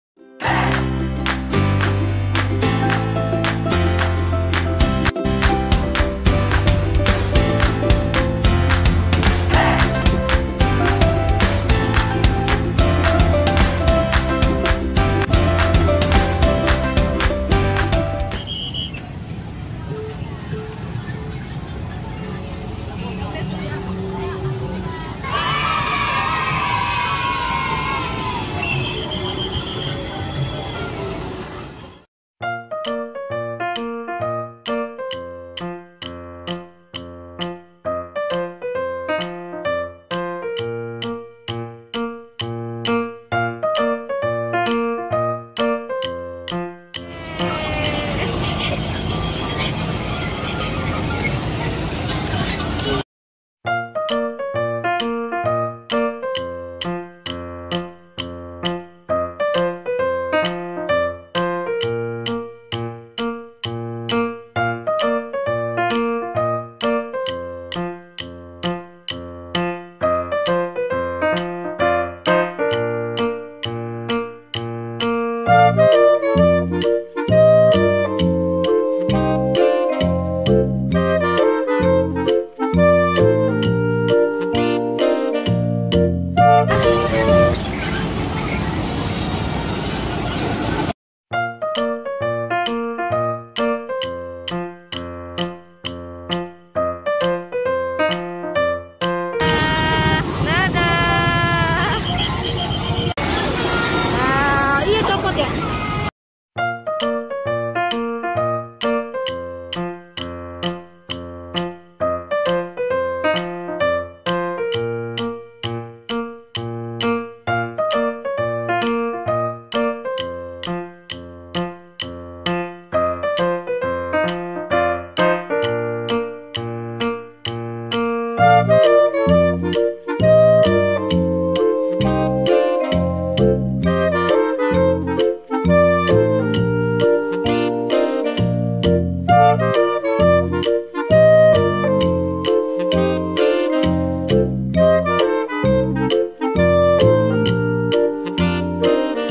SASA TEGANG PERTAMA KALI NAIK ODONG ODONG||TENSE TODDLER FIRST RIDES ON ODONG ODONG HORSE
📍DANAU CAVALLI tanah kusir jakarta selatan
harga 5k